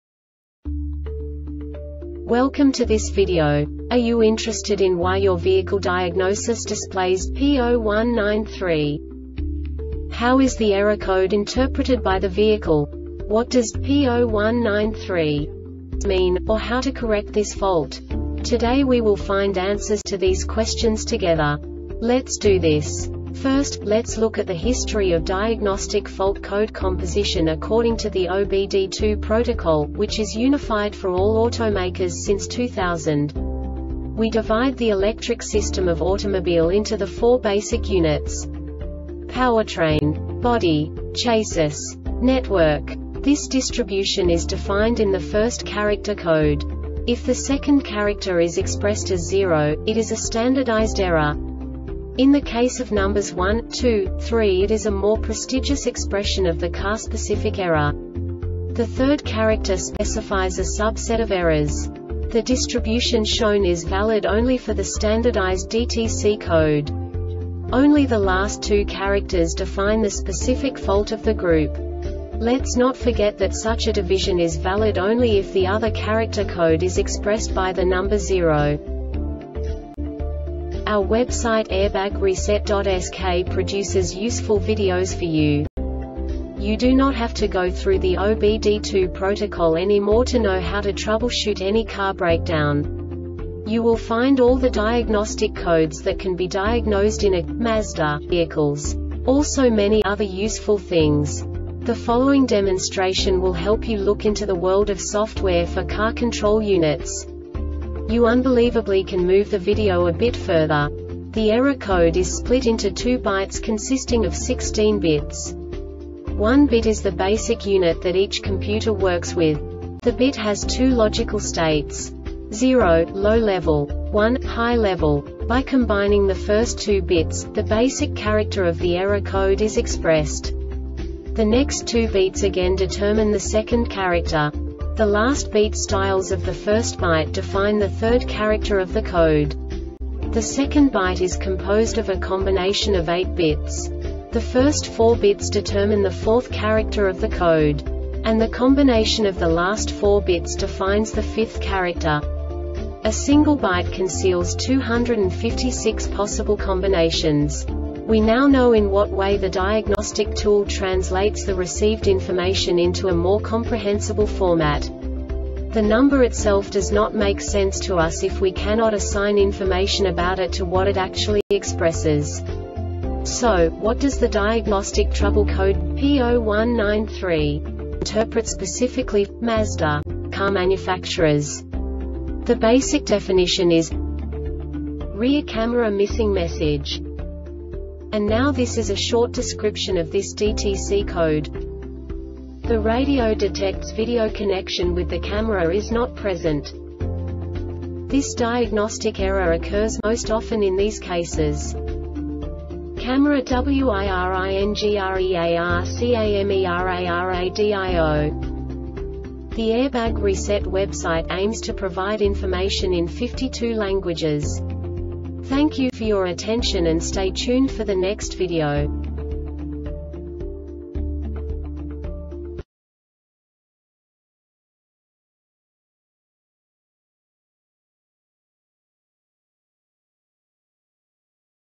DTC Mazda P0193 Short Explanation
Contents:
0:21 Basic DTC analysis according to OBD2 protocol standard.
1:48 Insight into programming
2:58 A diagnostically understandable description of the Diagnostic Trouble Code
3:05 Basic error definition

Make:
Mazda
Code:
P0193
Definition:
Rear Camera-Missing Message
Description:
The Radio detects video connection with the Camera is not present.
Cause:
CAMERA WIRINGREAR CAMERARADIO